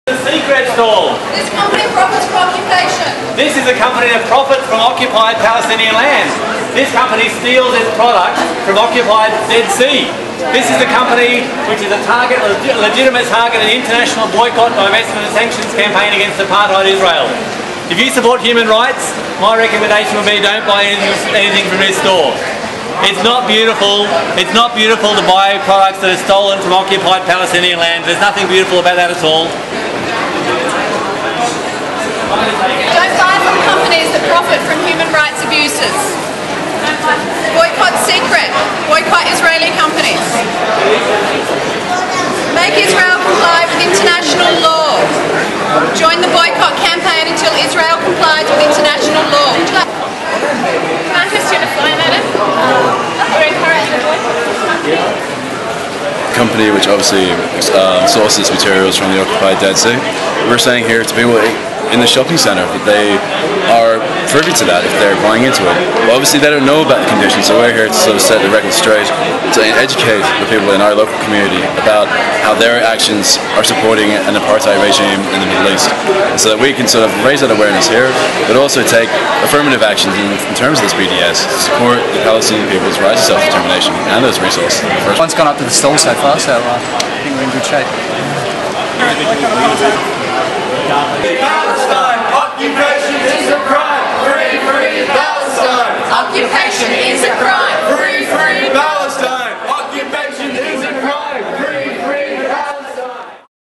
boycott, divestments and sanctions action 14 may perth 2011
An action by friends of palsetine western australia demanding a boycott of seacret cosmetics.

Seacret is an Israeli takes minerals from occupied Palestine land, without the Palestine people getting any of the money from it. The occupation of Palestine is in violation of international law, thus seacret is selling stolen goods and breaking international law.